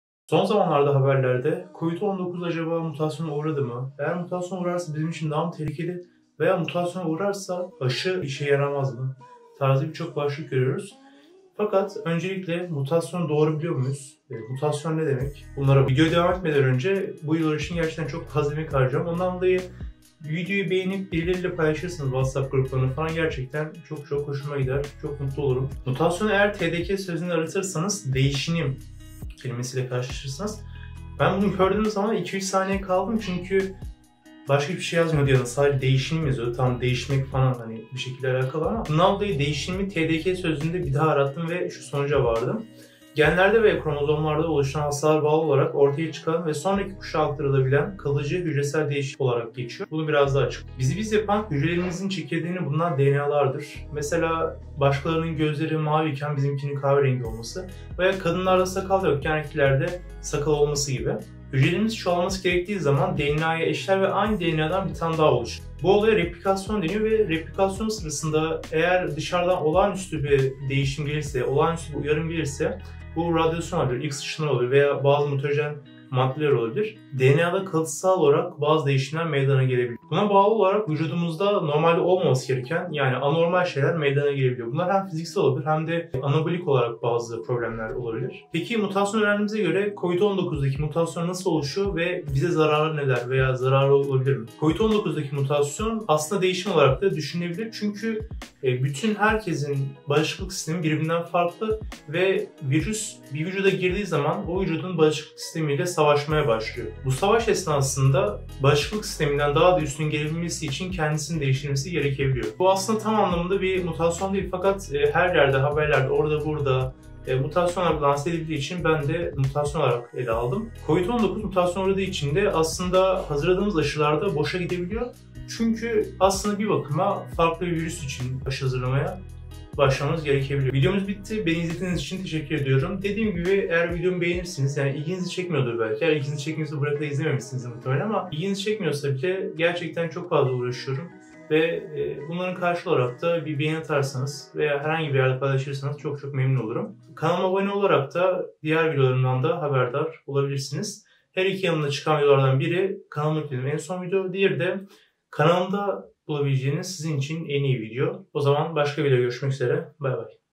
KORONA MUTASYONA UĞRADI MI? | MUTASYON NEDİR?
Özellikle bu aralar sağda solda çok fazla mutasyon haberi görmeye başladık, acaba mutasyonun gerçekten ne olduğunu biliyor muyuz? Korona gerçekten mutasyon geçiriyor mu yoksa farklı bir durum mu söz konusu? Koronanın mutasyona uğraması durumunda bize zararlarını ve mutasyonun ne olduğunu videoda konuştuk, iyi seyirler..

Okuduğum, izlediğim, öğrendiğim şeyleri sizlerle paylaşmayı seviyorum. Siz de kendinize bir şeyler katmayı seviyorsanız kanalıma göz atabilirsiniz :)